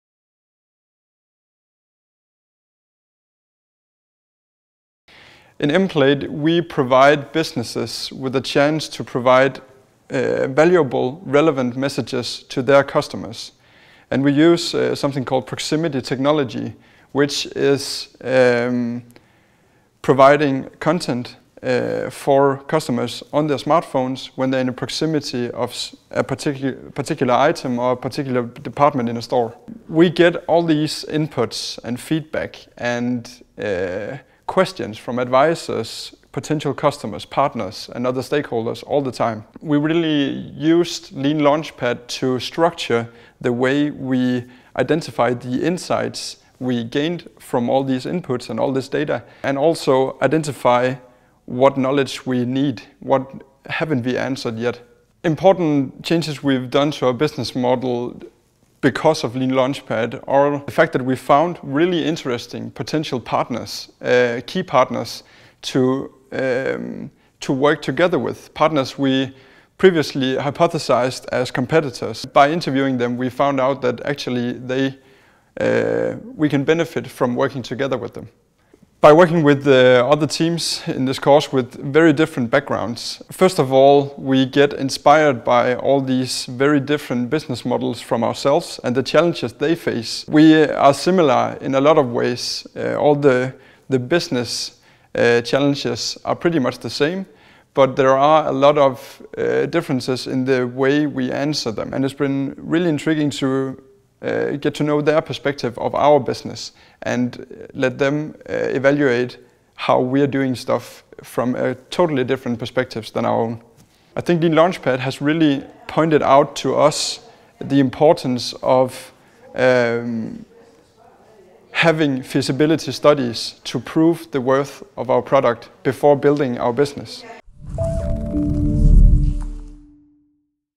Lean Launchpad: Emplate ApS
Kristian Lyhne, Partner in Emplate ApS, explains the value of participating in LeanLaunchPad course at Aarhus University.
The course is part of Science for Society project that enables PhD-students and postdocs in Scandinavia to commercialize research results as it equips them with the required entrepreneurial knowledge and “toolkits” to create impact in society

The LeanLaunchPad course consists of 7 workshops and are focused on business model design and customer discovery.

Emplate have specialized in the iBeacon-technology which makes location-based information on smartphones possible indoors.